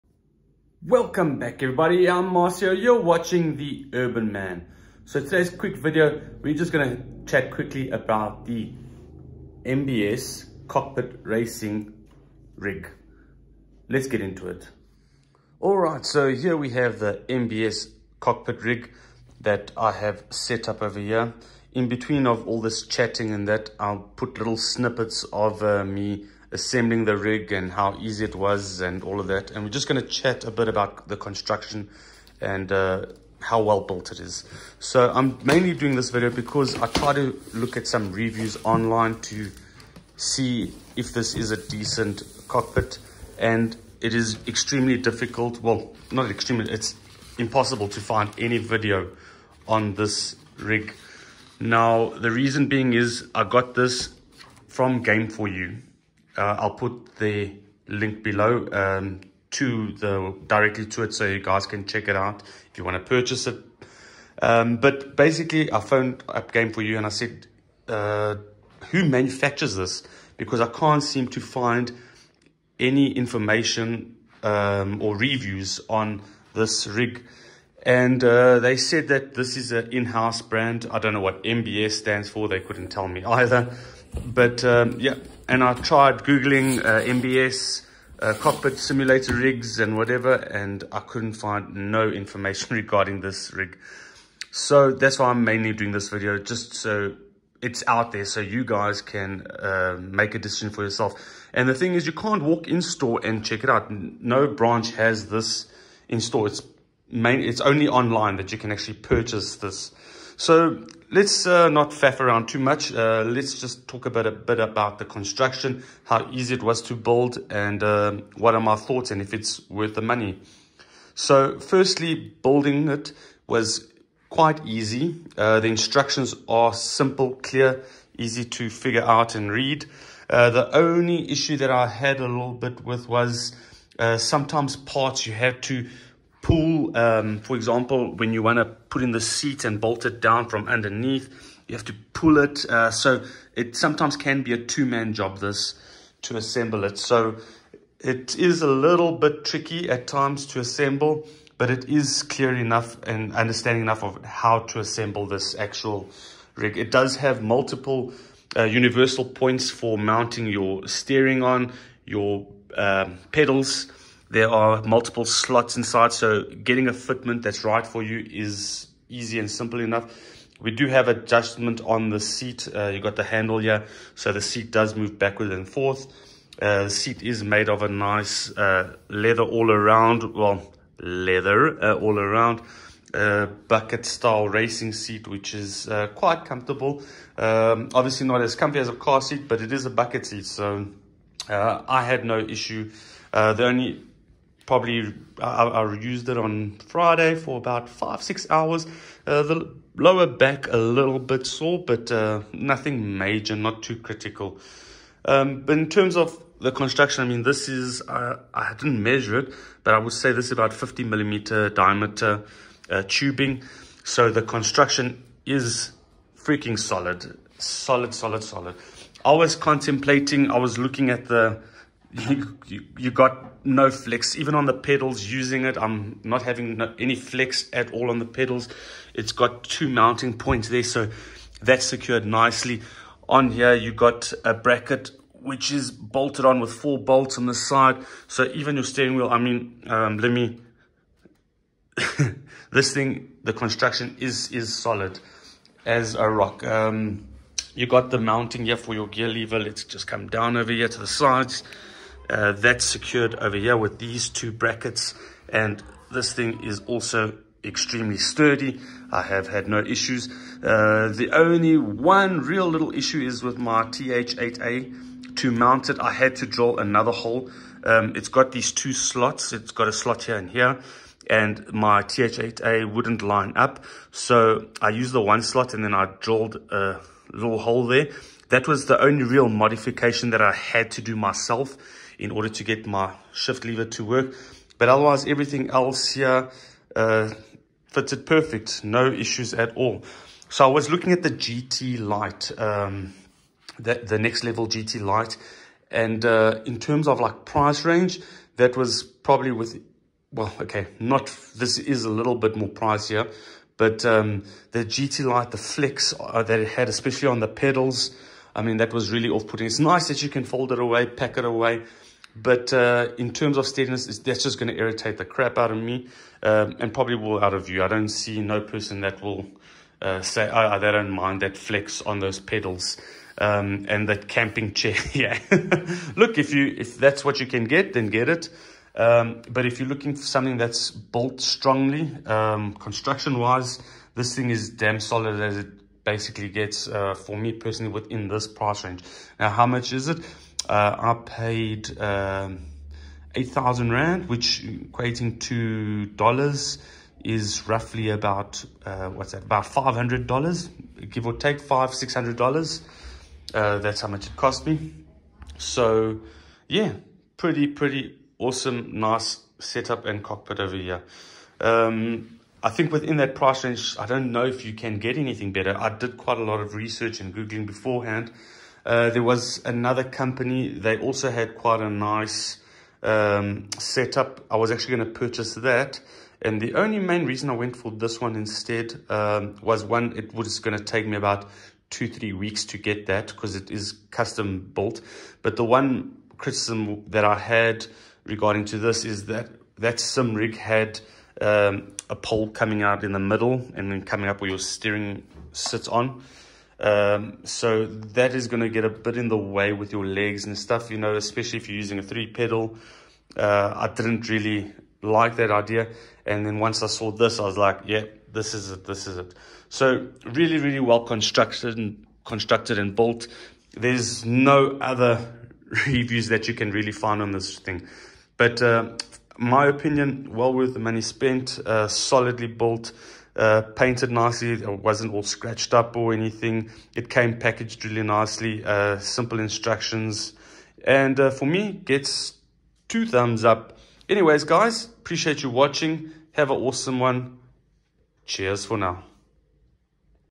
MBS Sim Racing Rig Overview
Todays video we take a look at the MBS Sim Racing Rig Cockpit.